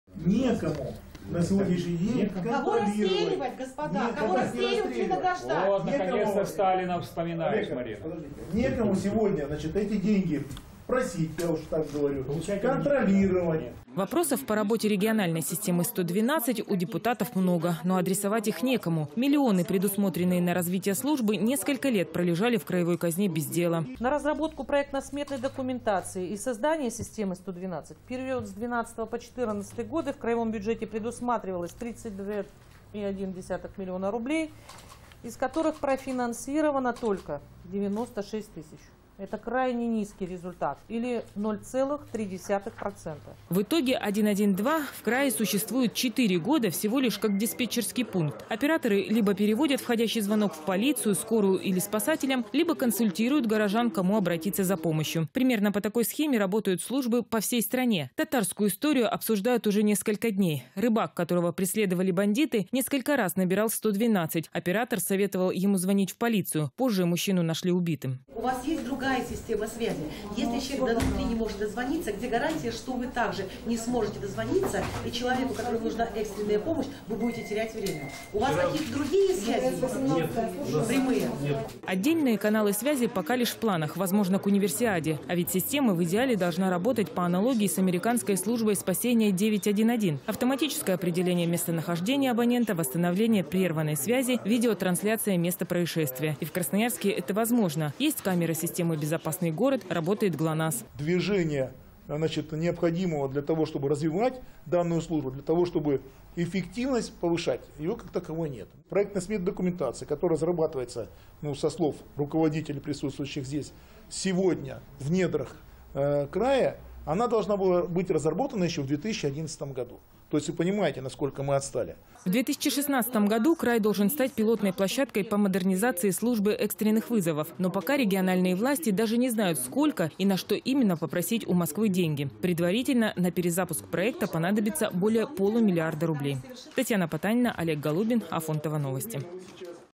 Служба 112 не работает. Новости Афонтово. 20.10.15
911 по-красноярски. Краевые депутаты проверили, как функционирует служба экстренных вызовов 112. И обнаружили, что система не работает. Если горожанин не может дозвониться в скорую, операторы,  к сожалению, не помогут: для них  линия также  занята. Почему миллионы, предназначенные на развитие службы 112, никто не использовал, и заработает ли она к Универсиаде, узнавала Татьяна Патанина.